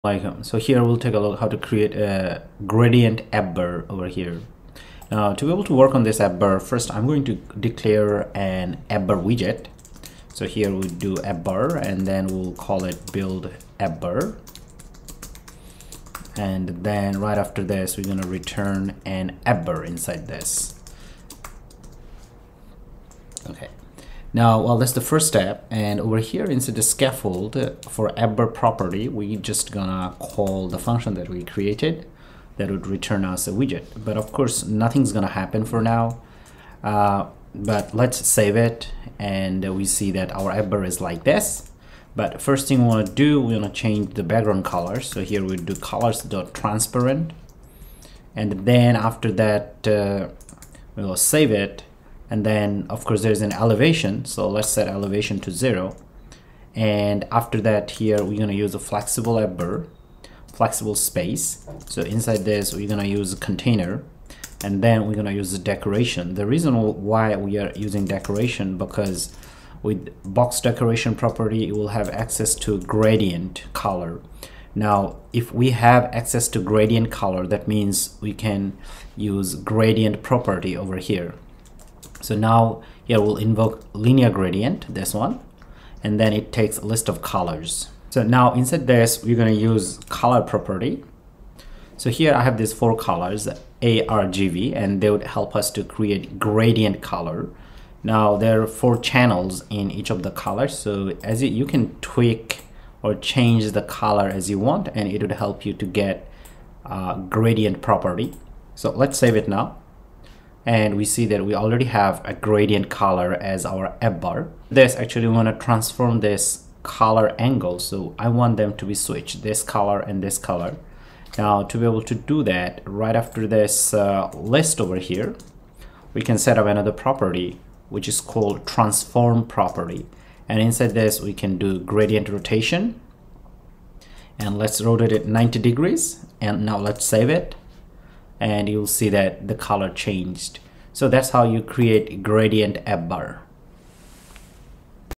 Flutter Gradient AppBar
Learn how to make gradient app bar in flutter using lineargradient